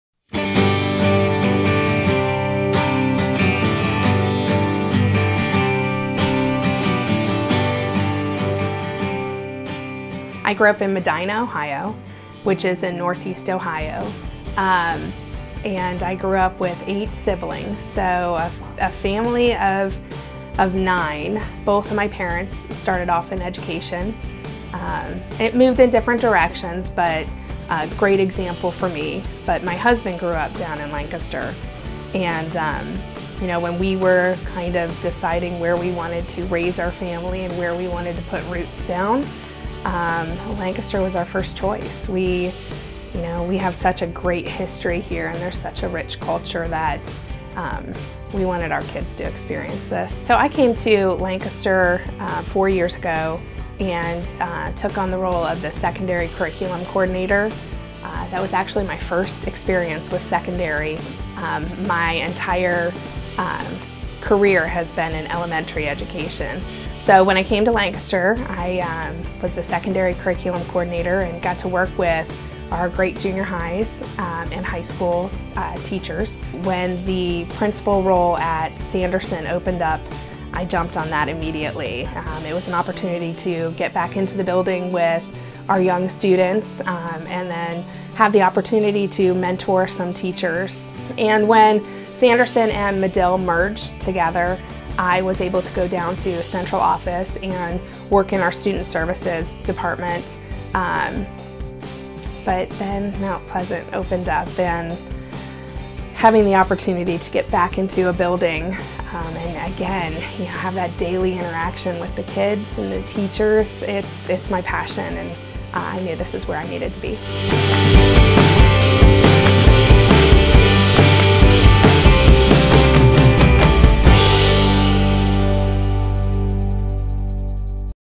Meet New Mt Pleasant Principal Shannon Burke
Shannon Burke has worn many different hats during her four years at Lancaster City Schools, and next year she will take over as the principal at Mount Pleasant Elementary!